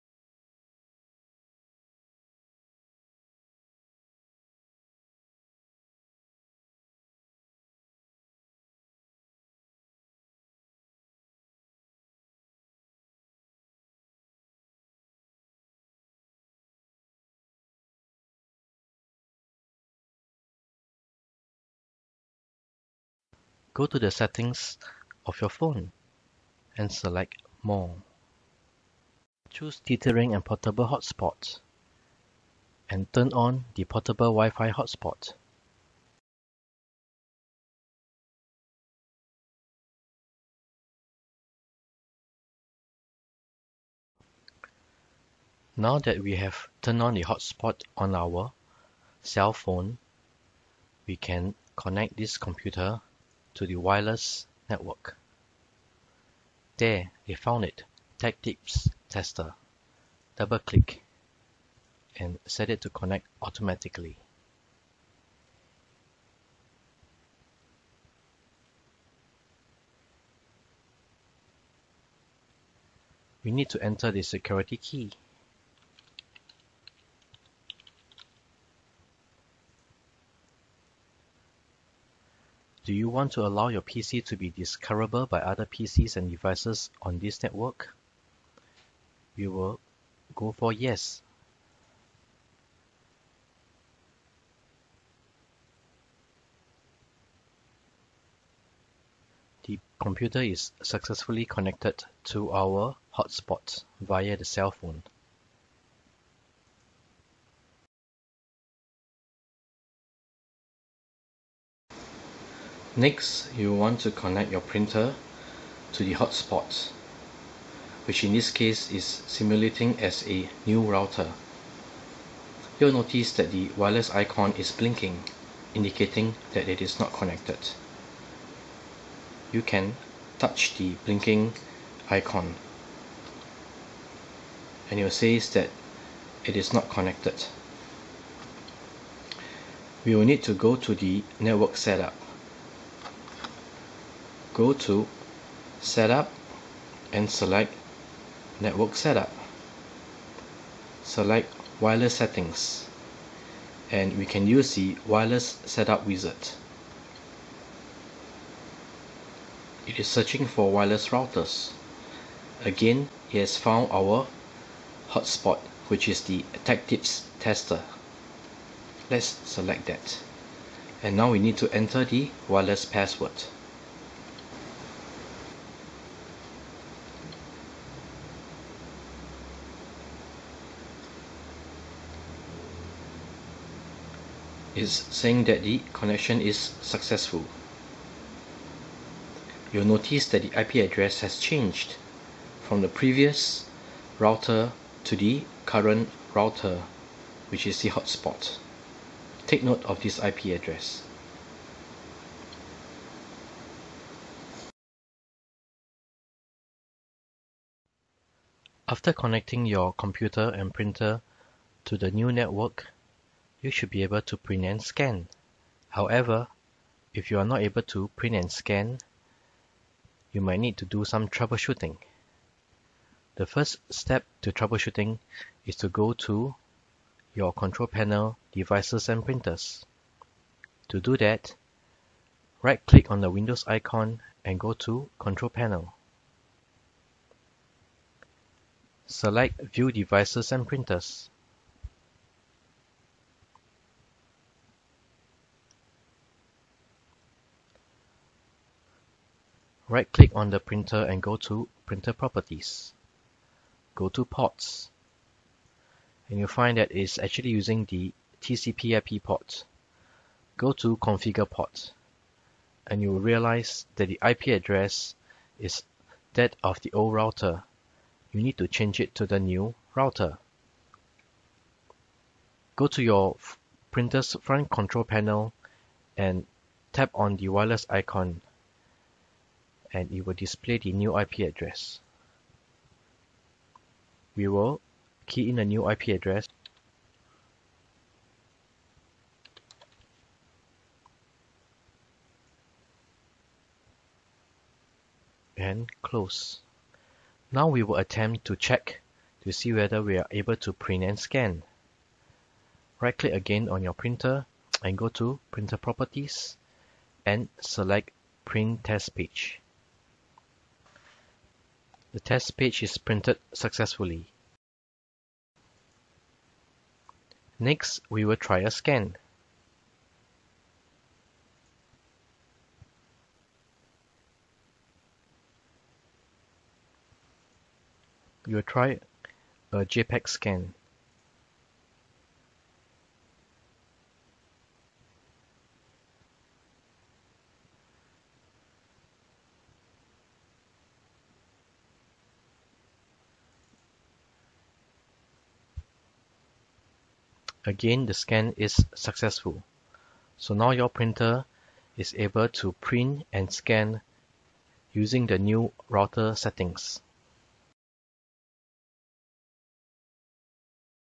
HP Officejet Pro 8710 | 8720 | 8730 | 8740 : Changing to a new router and using the printer
This tutorial shows you how to connect your printer to a new router without reinstalling the software.

We used a cell phone hotspot to simulate a new router. Note that your computer must be connected to the same hotspot as well,

It also shows the basic troubleshooting in terms of checking the ports used.  The tutorial shows how to change the IP address of the TCP/IP port that was used in the previous router but has changed with the new router and demo a successful Print and Scan using the new router.